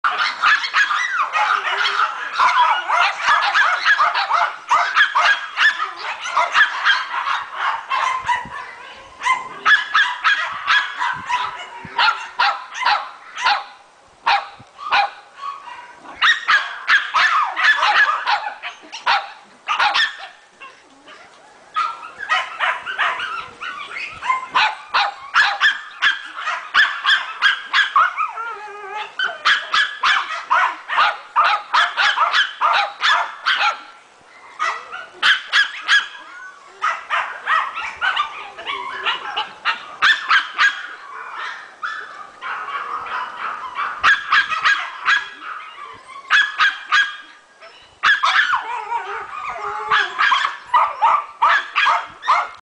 Maltese puppy - Toko
Sweet and playful Maltese puppy.
Toko is a tiny size Maltese boy